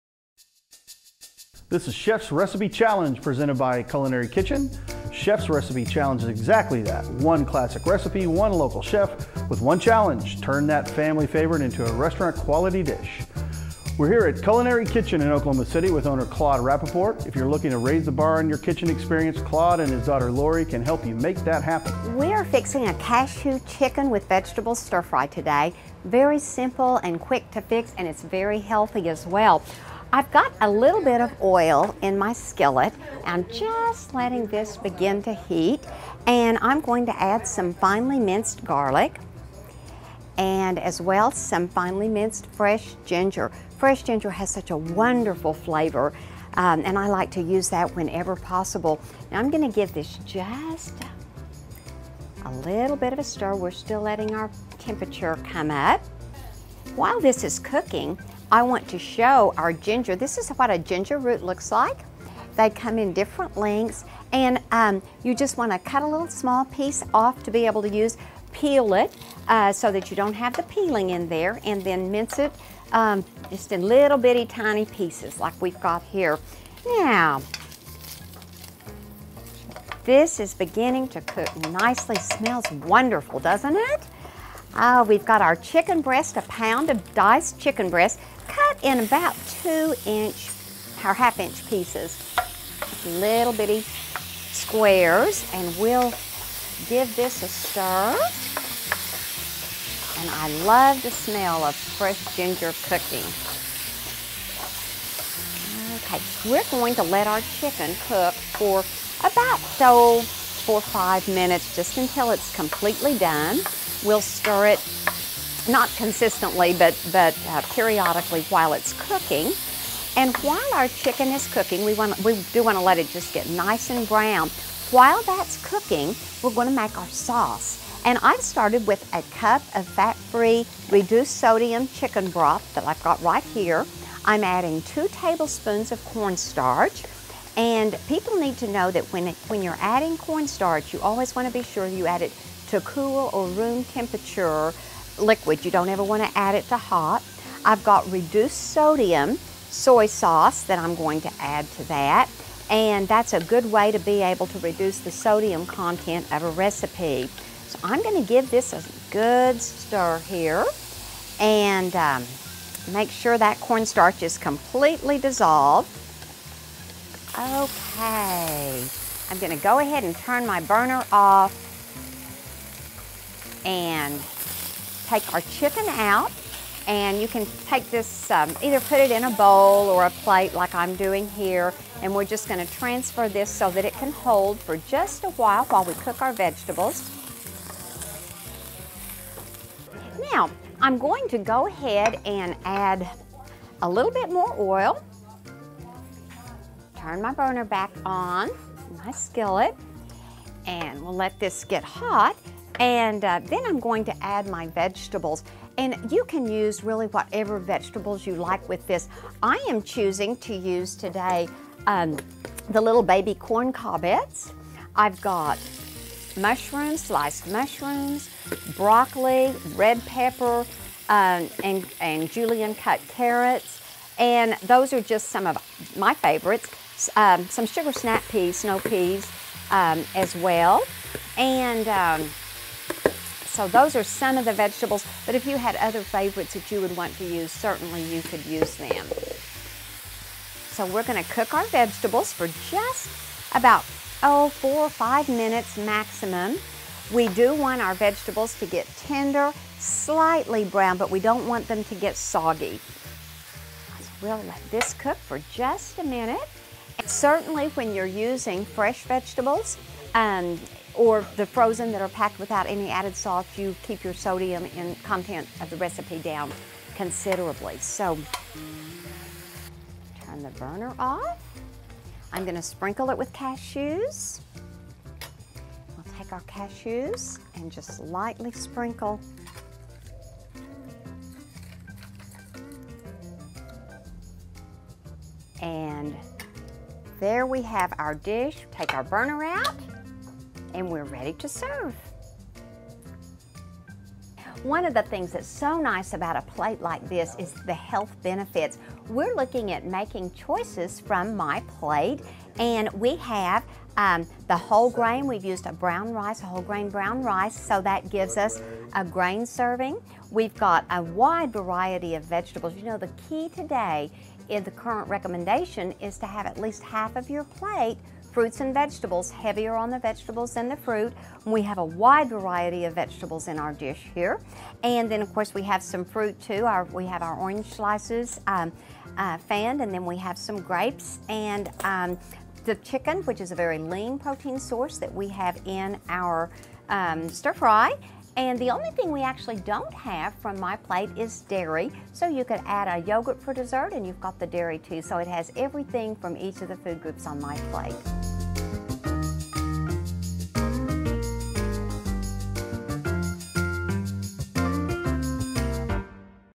Becky Varner: Stir Fry (2012-01-17)
Cooking stir fry with Becky Varner.